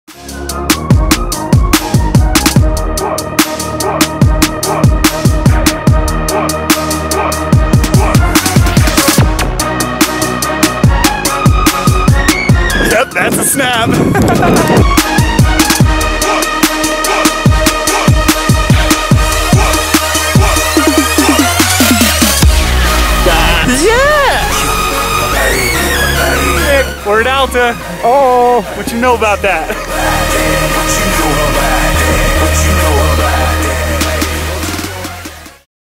April 24, 2016 - 13" Of Pow!
Cancel your golf game, it's a powder day at the 'Bird! We received 13 inches of new snow in 24 hrs. and 11 inches in 12 hrs. Happy spring skiing!

Tag along with Brooks Finlinson while he shreds that sweet, sweet late season pow.